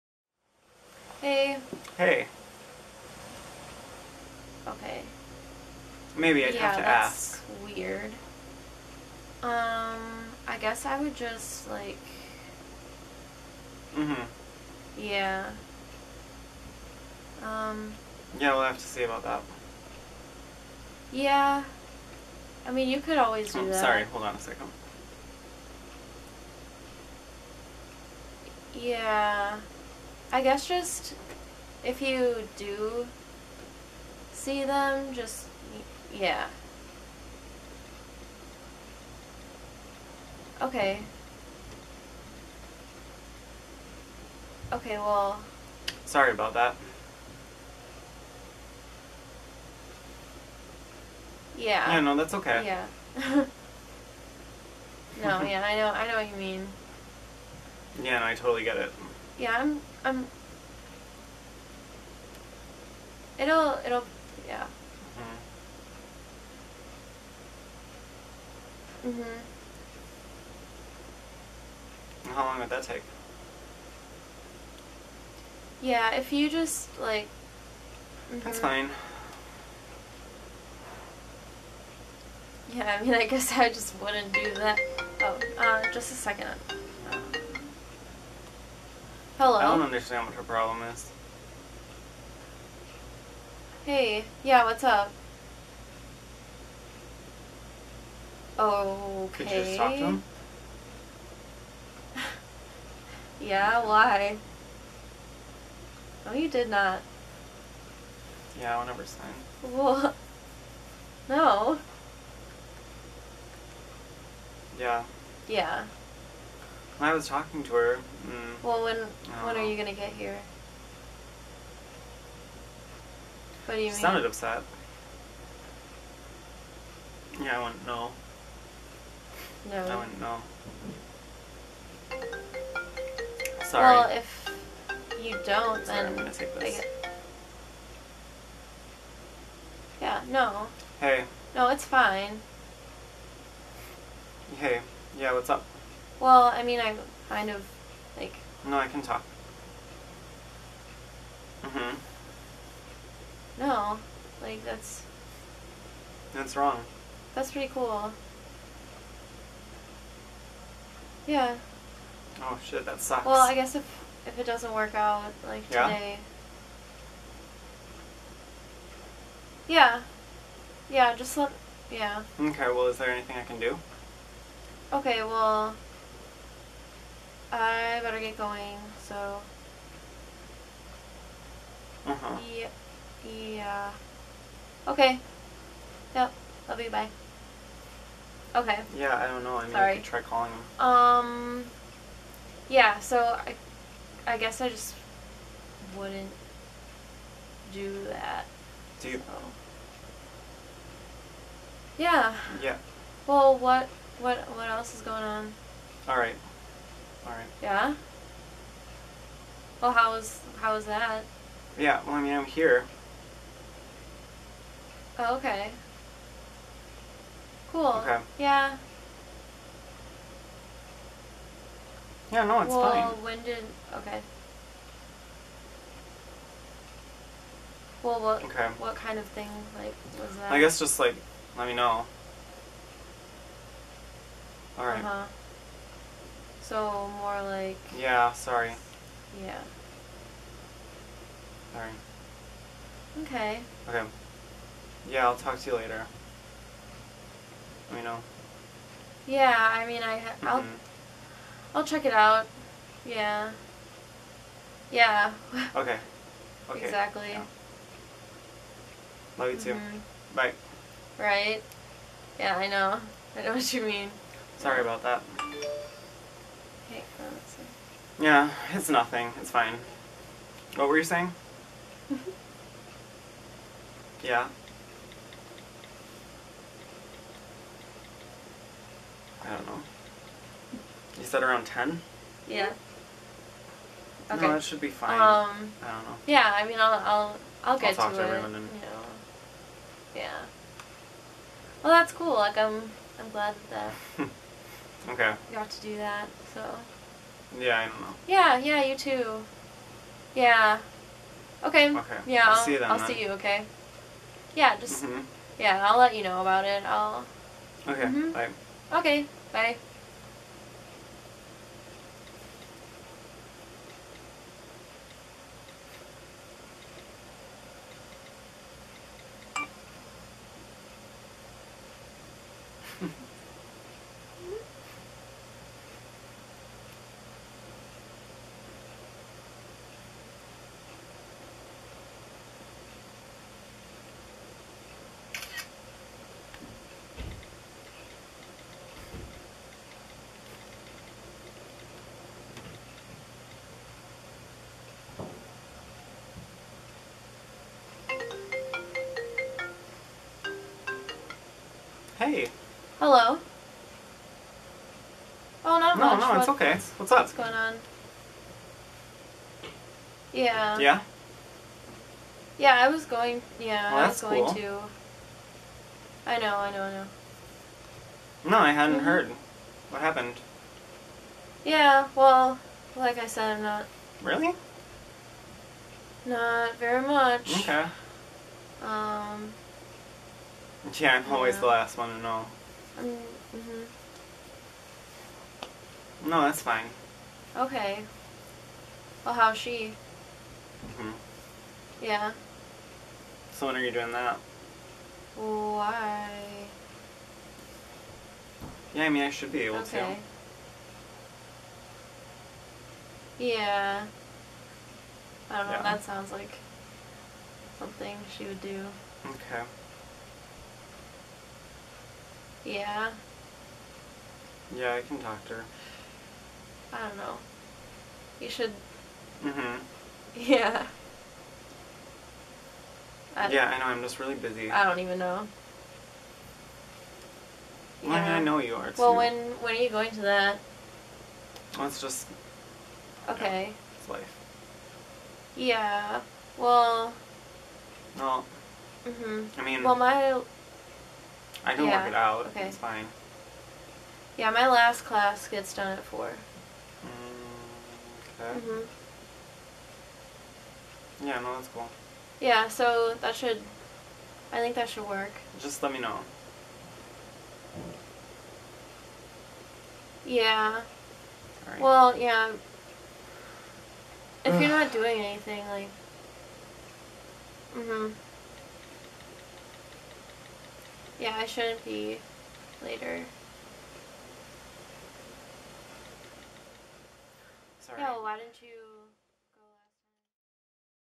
Conversations - Extended
Conversations is a multichannel video performance exploring modern communication and the ways we interact with one another. My collaborator Molly Surber and I, the performers, have one-sided conversations using dialogue taken from previous recorded interactions we have had. Technology interrupts our recording, causing stuttering and confusion. The video  frames the degradation of verbal dialogue and memorializes seemingly mundane conversations as important. In doing so, it asks viewers to look introspectively at how modern technologies are affecting their own engagements with one another.